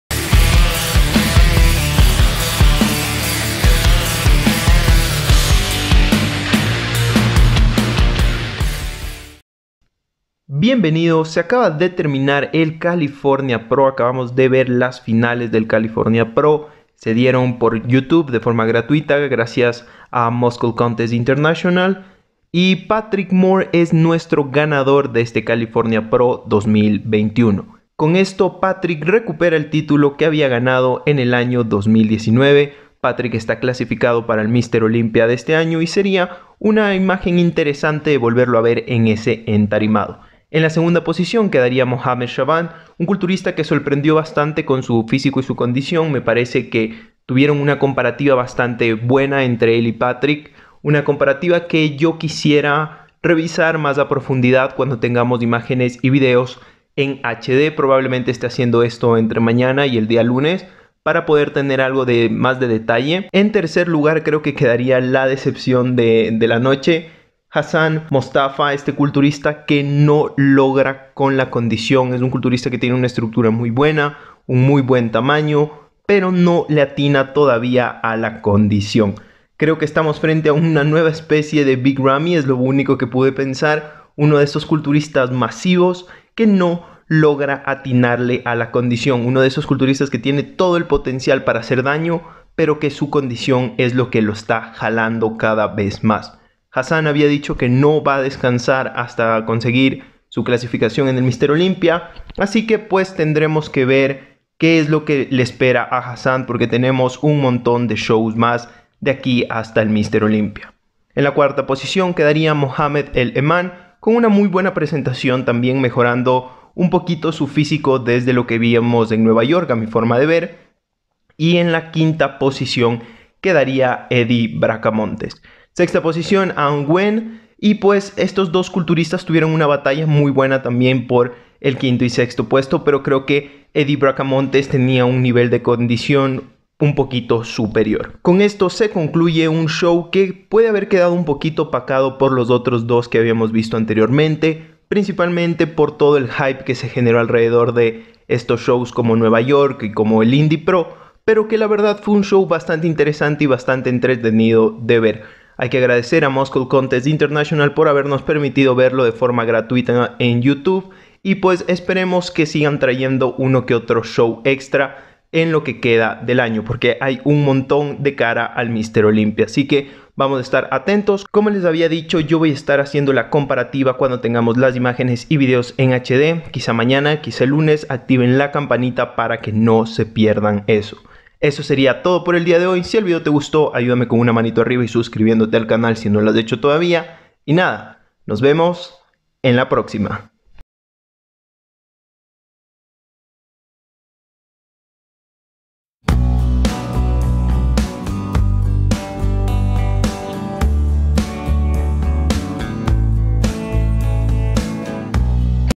Resultado Finales California Pro 2021. | Patrick al Mr. Olympia 2021 y Hassan vuelve a decepcionar.
¡NUEVO VIDEO!